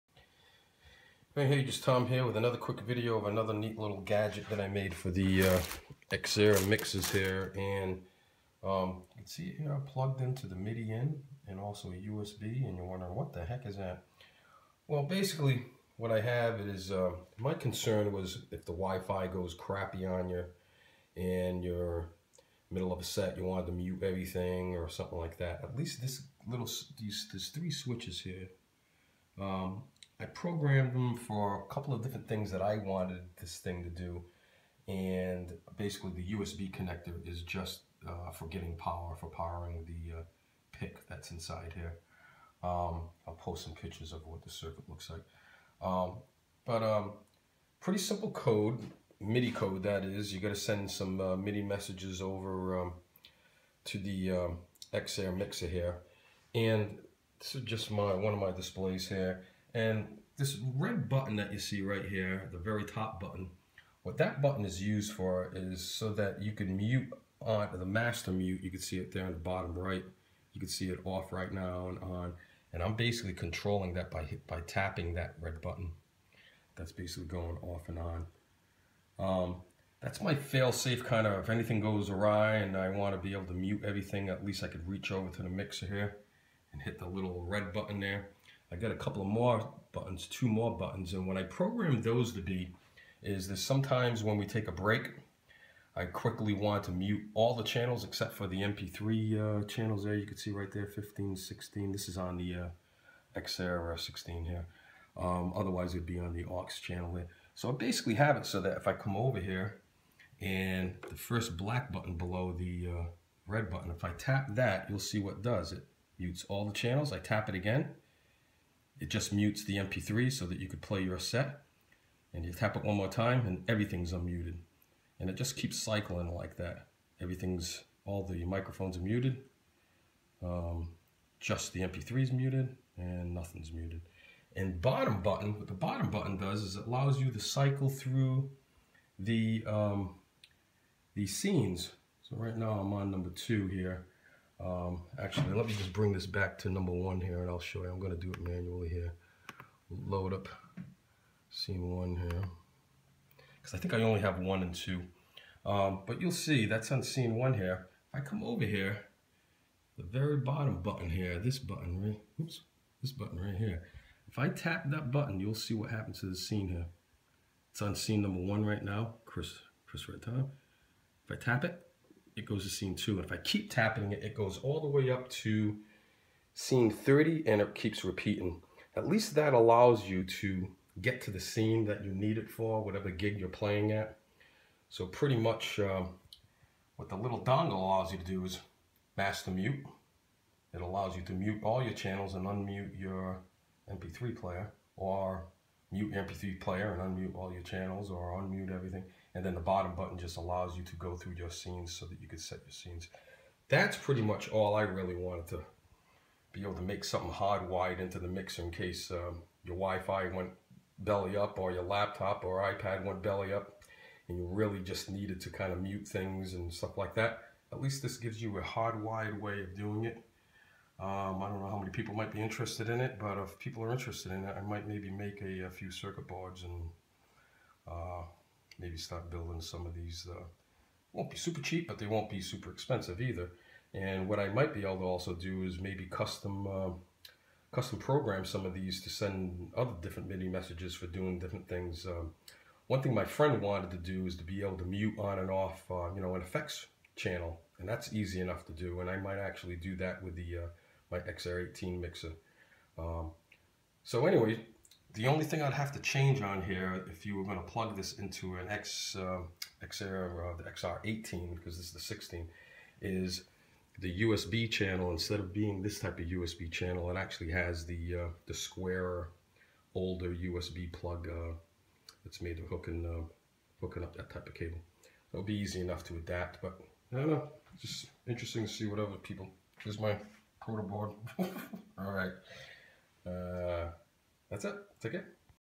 Behringer X-Air XR-16 or XR-18 Digital Mixers MIDI Dongle Control
This is a cool little device that plugs into your Behringer X-Air digital mixer (XR-16 or XR-18)… It is a pic-based controller with three switches which allows you to easily plug into the midi input port on your mixer, allowing you to control certain functions such as Fader levels, L/R balance, effects, mutes, etc...